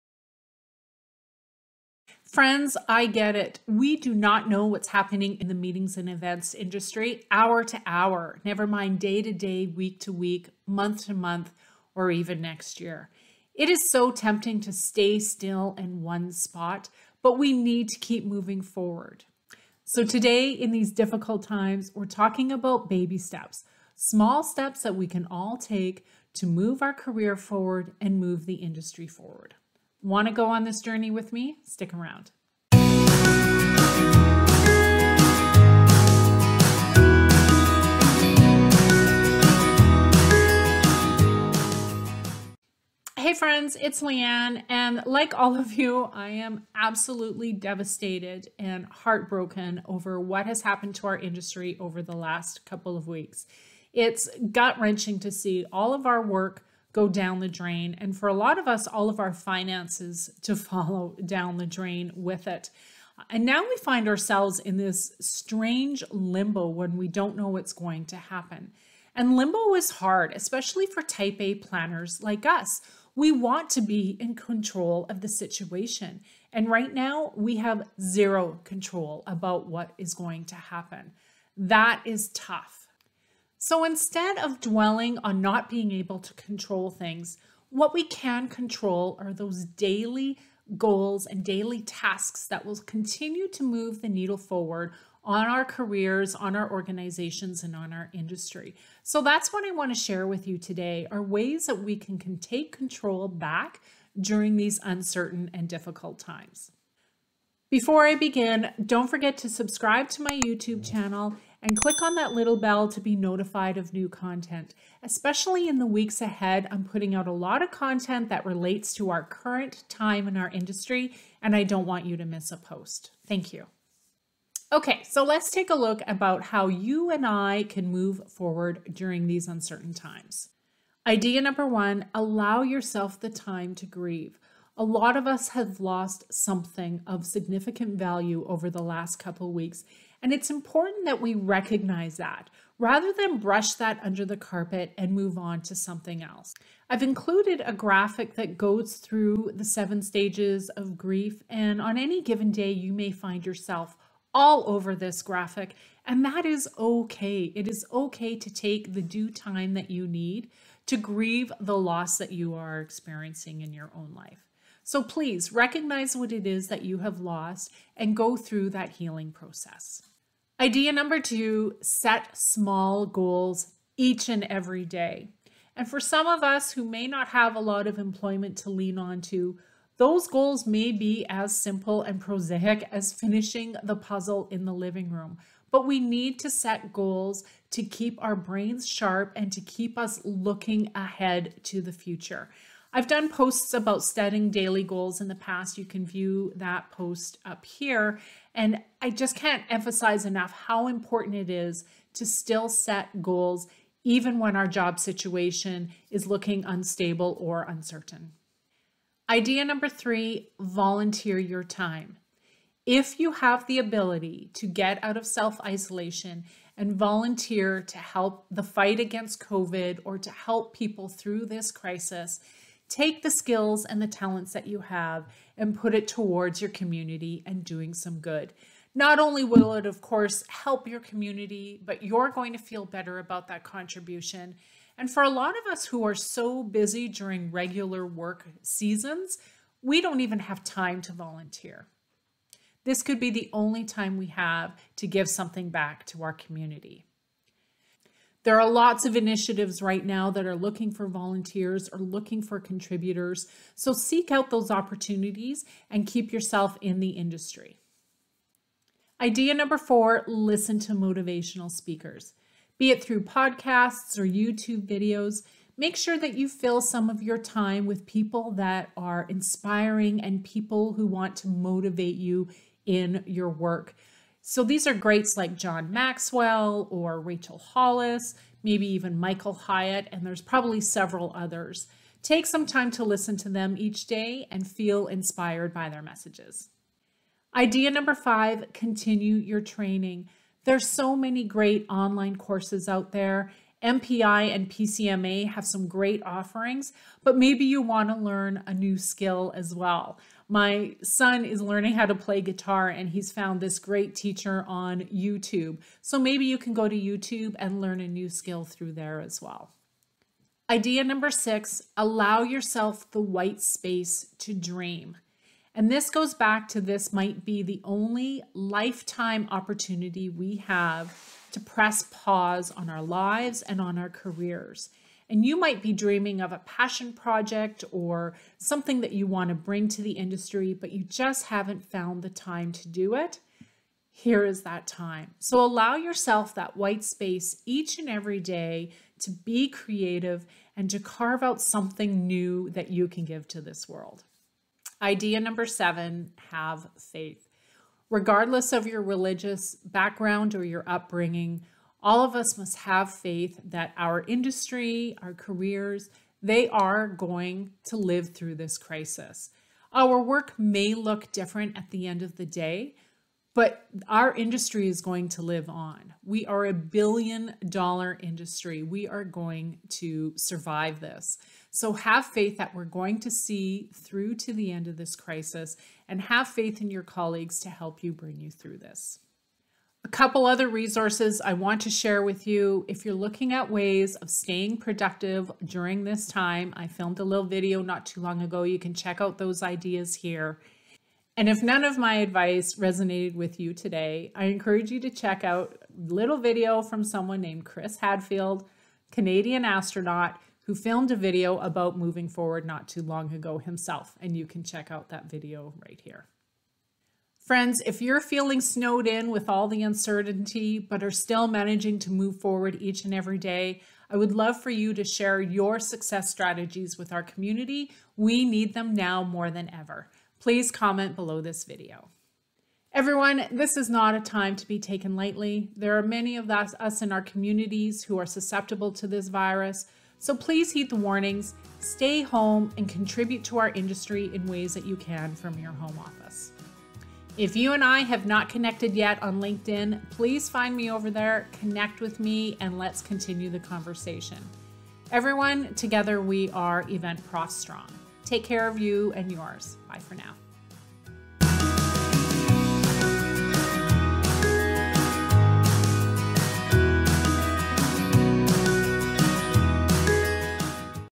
7 Ways You Can Move Forward In Times of Uncertainty in the Meetings Industry
Friends, whether an eventprofs or a hotelprofs, your life has been turned upside down over the last few weeks with covid19. I have run the gamut of emotions the last few weeks, but I’m finding solace in the belief that it will be ok.

Are you needing some encouragement or advice today on how to move forward during this time of uncertainty?